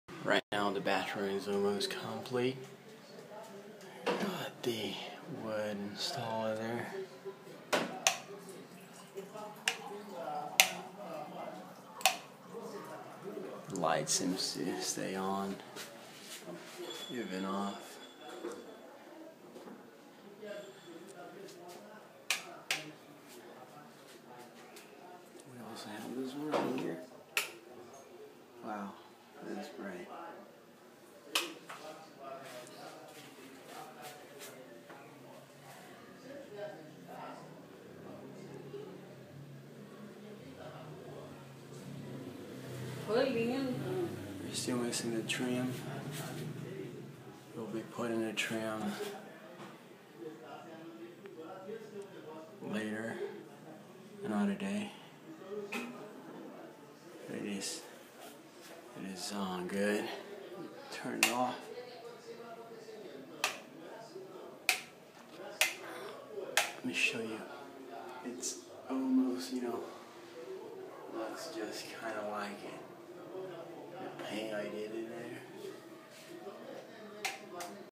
Build new wooden ceiling on bathroom
Used a Makita af505 brad nailer with nails 30mm 18gauge